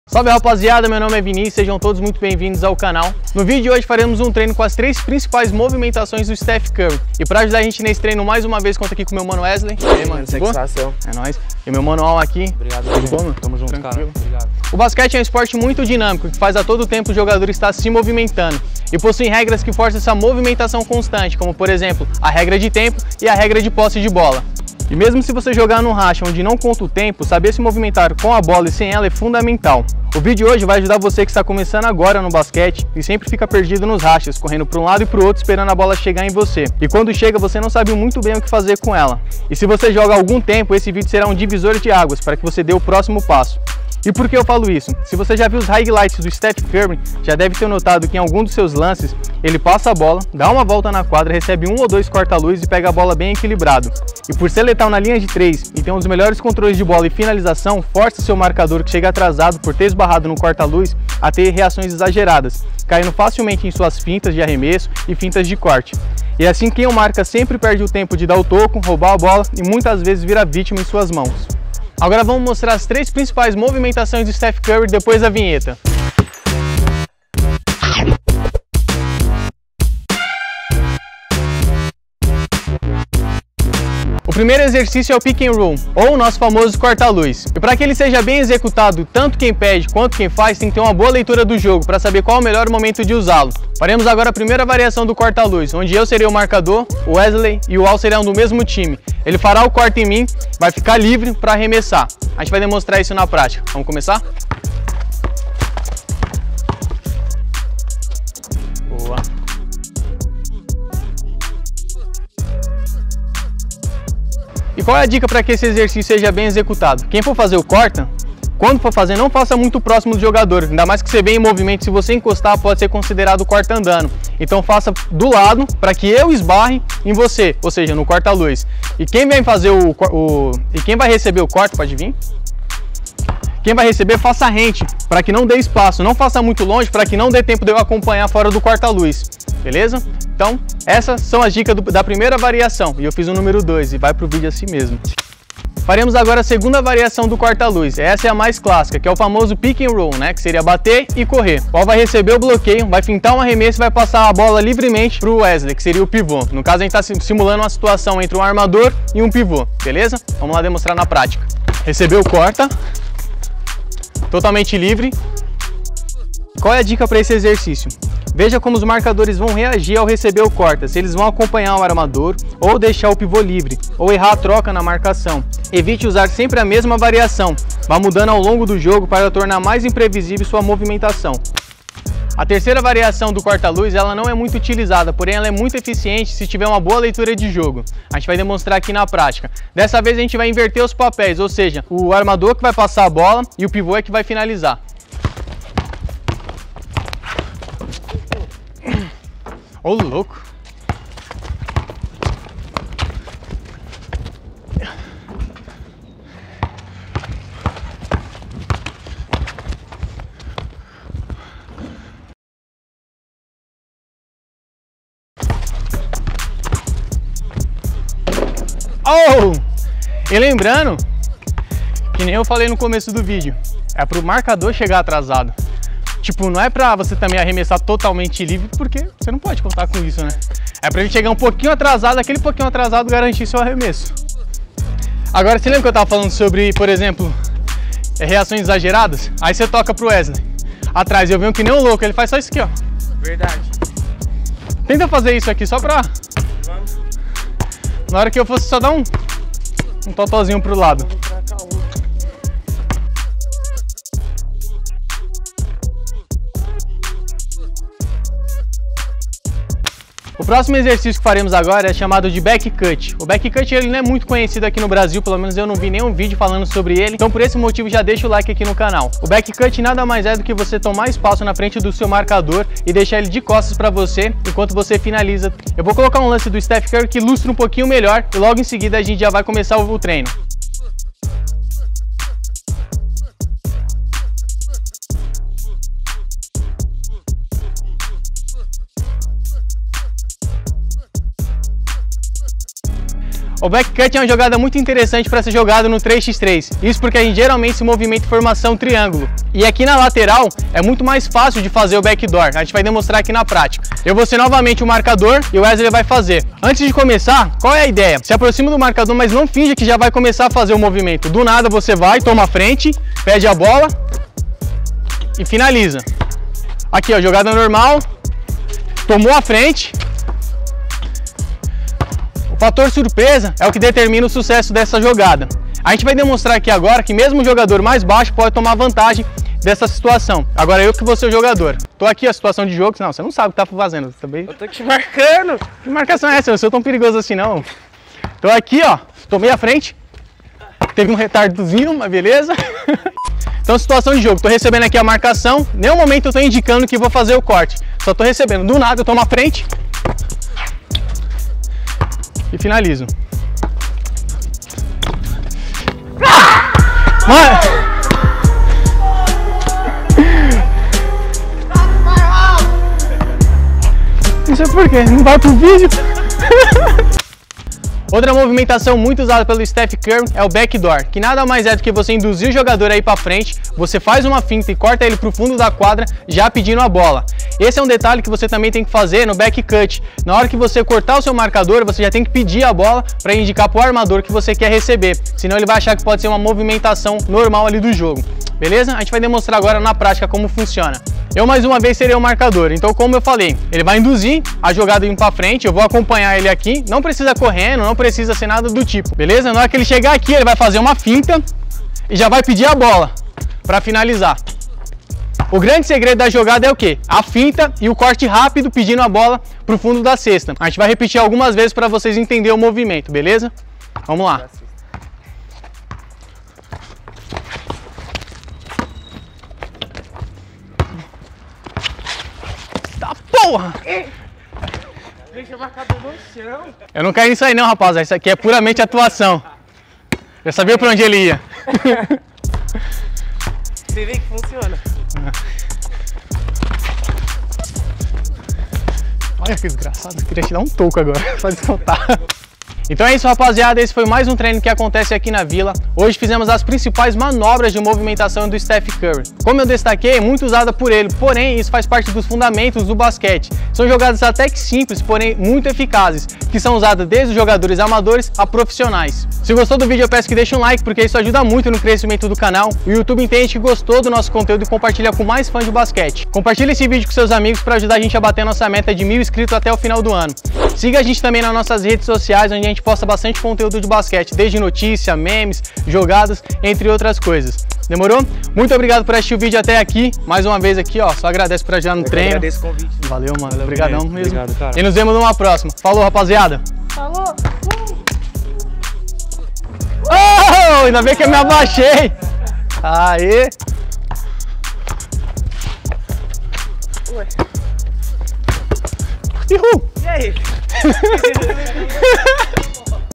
O Segredo da movimentação do Stephen Curry - Como fazer Pick and roll e corta-luz no basquete
Salve galera, o basquete é um esporte realmente muito dinâmico, temos que correr, saltar, ter o controle de bola, tudo que fazemos do lado direito que temos fazer do lado esquerdo. Mas não basta somente dominar os fundamentos de controle de bola e finalização para dar o próximo passo em busca de ser um atleta profissional; para jogarmos em auto rendimento temos ter inteligência de jogo para sabermos qual melhor momento de fazer um corta-luz, qual melhor jogada usar em cima do time adversário, saber fazer uma boa leitura de jogo para chamar um jogada de back door com seus companheiros de time. E um dos jogadores da atualidade que faz isso como ninguém é Stephen Curry, que além de ter uma inteligencia de jogo incrível, se movimenta dentro de quadra de forma organizada, deixando seus marcadores sempre confusos, fazendo assim deles vitimas em suas mãos, para matar bola de 3 pontos, infiltrar usando o pick and roll, saindo de jogadores mais altos com back cut. Então não deixe de assistir esse vídeo até o final e saber também o segredo por trás da movimentação do Stephen Curry.

Kwai: baska de vila oficial

* Agradecimentos especiais aos meus mano Wess e Wall que acordaram de manhã cedo e treinaram comigo de baixo do sol de 30º